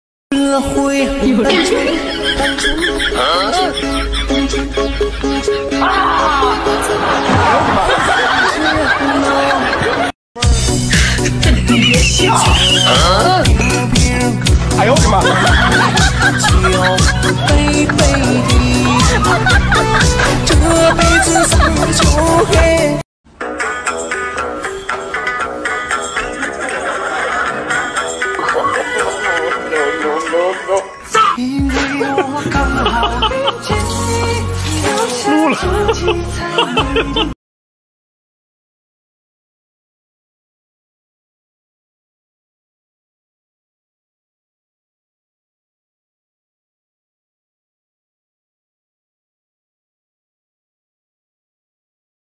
Top Funny Chinese Video Compilation | Episode 13
Let's enjoy a laugh! Among these three videos, choose the one that brings the most humor.